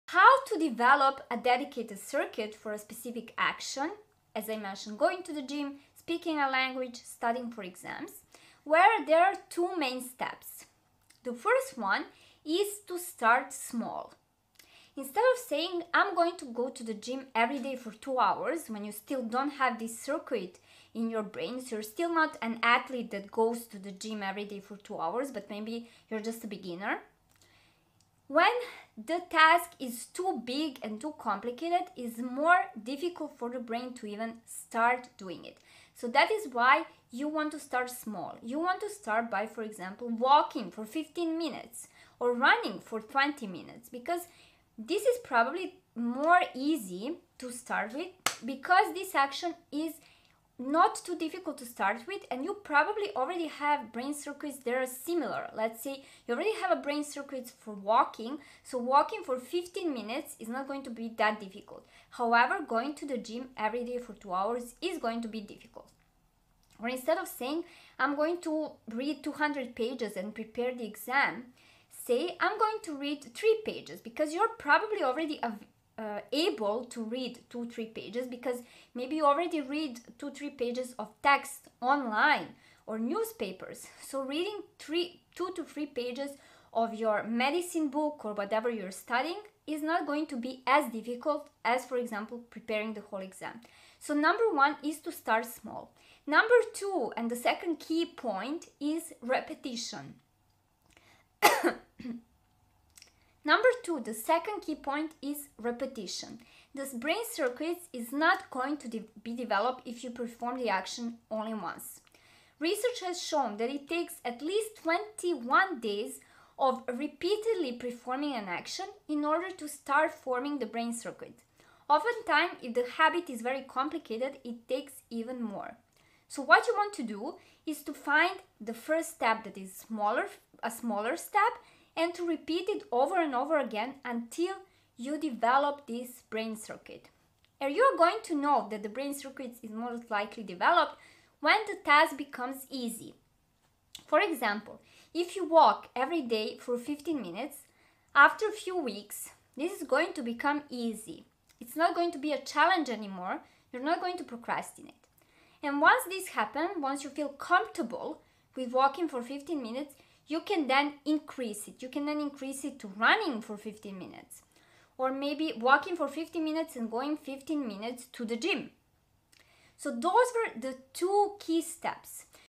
Two Simple Neuroscience Steps to Beat Procrastination Starting Today!
💭 You want to work out, build your business, or create content — but instead, you clean the kitchen, scroll your phone, or go out with friends. Sound familiar?

You’re not lazy — your brain just hasn’t built the circuit yet.

🧠 In today’s video, I’ll explain how neural wiring affects procrastination — and how to train your brain to make habits automatic and effortless.

✨ You’ll learn:

1️⃣ Why procrastination means your brain hasn’t formed the right circuit yet

2️⃣ How to build that circuit step by step using neuroscience-backed methods

3️⃣ The 2-step system to finally stop delaying important tasks

💡 Try this:

Start small. Work out for 15 minutes, build your business once a week, or go on one date a month — and repeat until it feels easy. That’s how the brain wires habits for good.

⏱️ Chapters

00:00 – Problem

02:24 – Step 1

03:52 – Step 2

05:13 – Summary